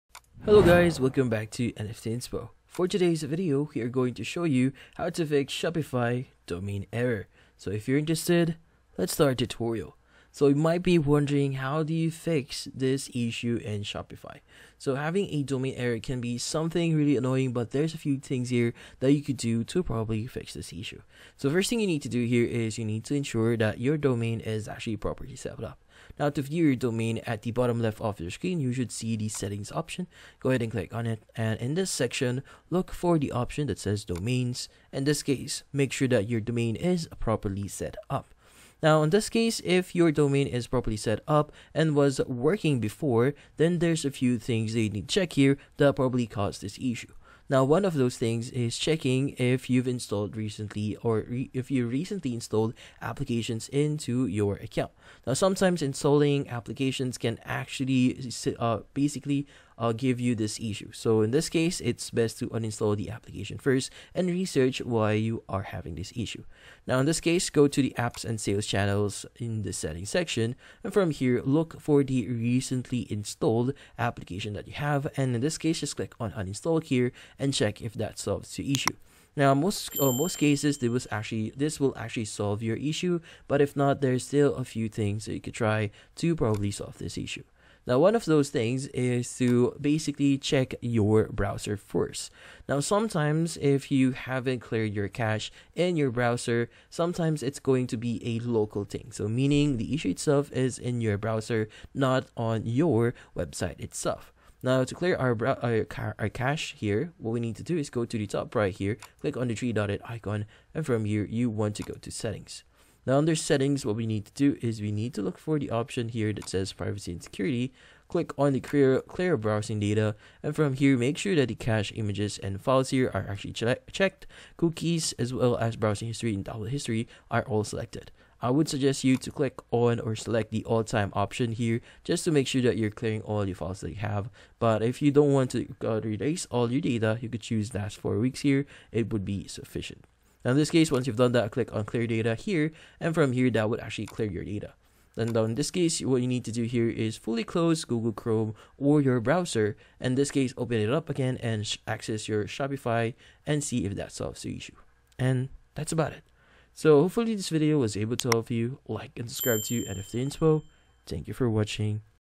How To Fix Shopify Domain Error (2025)
If you're encountering a domain error on Shopify, this tutorial will guide you through troubleshooting steps to resolve the issue.